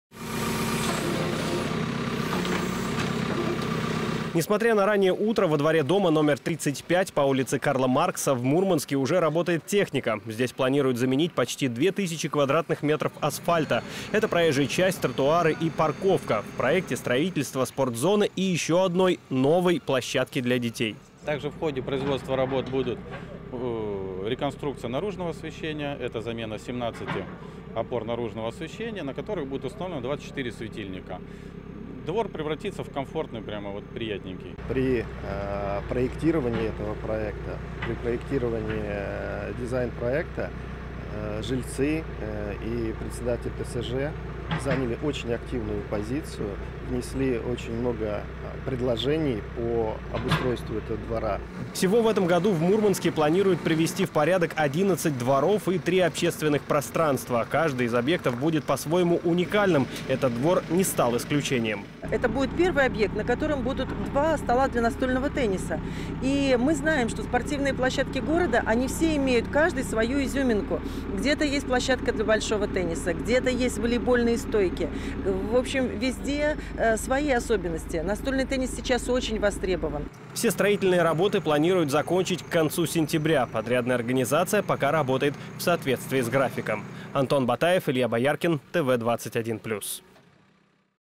В Мурманске создадут зону для настольного тенниса
Проект – часть реконструкции двора у дома №35 по улице Карла Маркса.
Решено, что каждая спортплощадка должна иметь свою изюминку. А еще во дворе приведут в порядок проезжую часть, тротуары, парковку, освещение. Там должна появиться также новая игровая детская площадка.
Все работы строители планируют закончить к концу сентября.